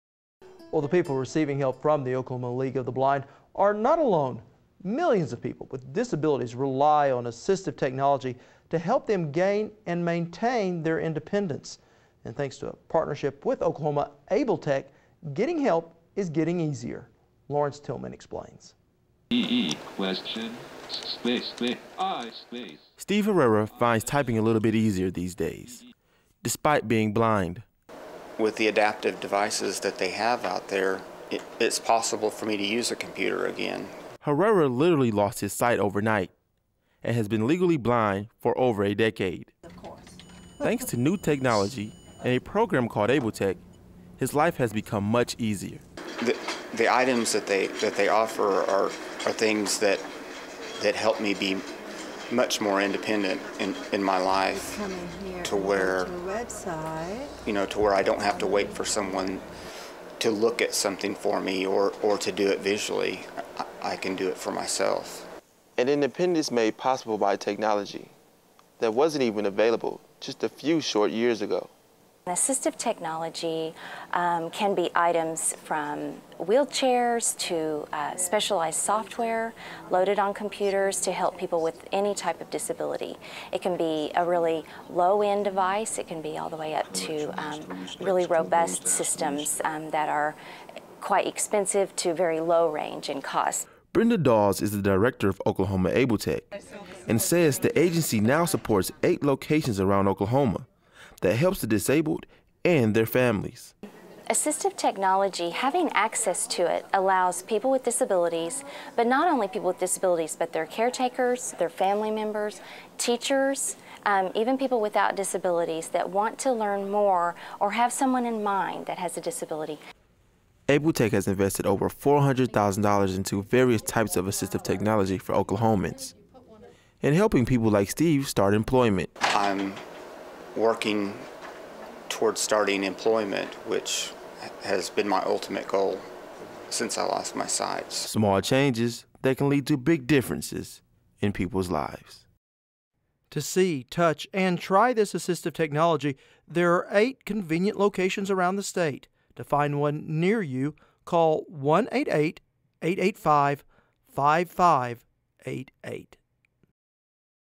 ABLE Tech enabling the disabled to be independent
Millions of people with disabilities rely on assistive technology to help them gain and maintain independence and thanks to a partnership with Oklahoma ABLE Tech, getting help is getting easier.